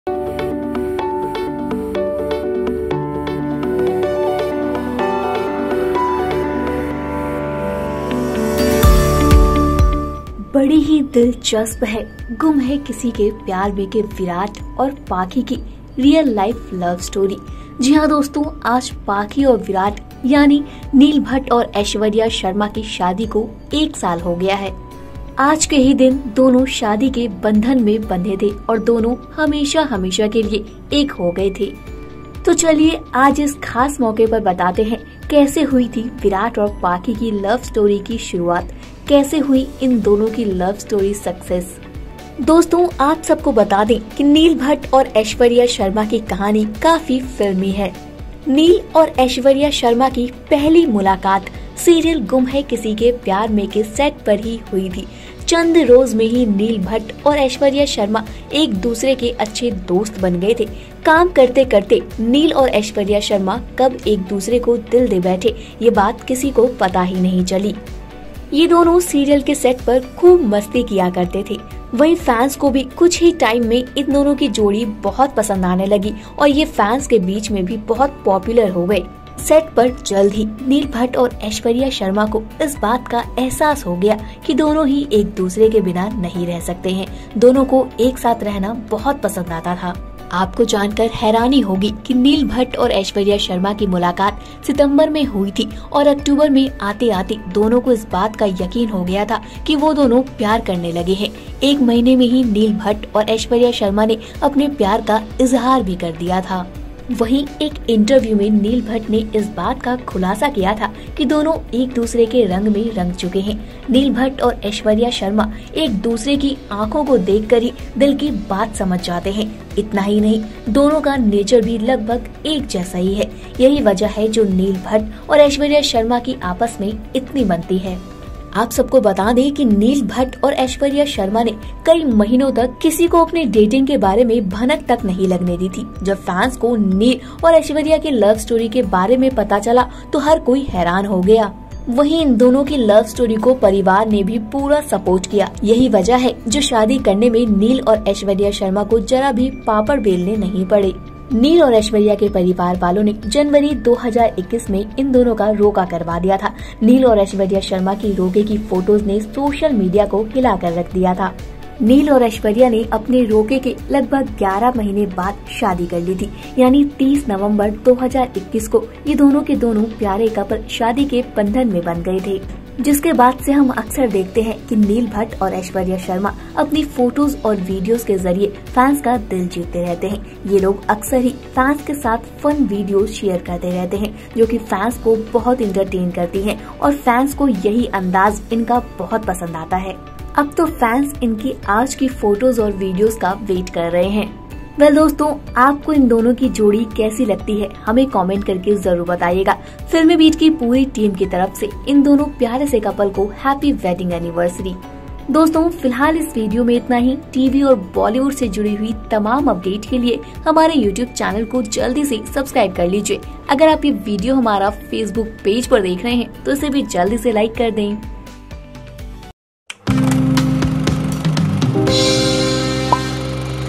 Gum Hai Kisi Ke Pyar Mein Fame Aishwarya Sharma और Neil Bhatt की शादी को हुआ एक साल,देखिए Love story
Gum Hai Kisi Ke Pyar Mein Fame Aishwarya & Neil Bhatt completed one year of marriage, See Love story. Aishwarya Sharma and Neil Bhatt are one of the most talked about couples in the telly industry. The couple, who had met on the sets of Ghum Hai Kisikey Pyaar Meiin, tied the knot in November last year, and ever since then, they have been dishing out major relationship goals. Neil Bhatt and Aishwarya Sharma both are very cute couple. Neil Bhatt and Aishwarya Sharma doing fun in new fun videos.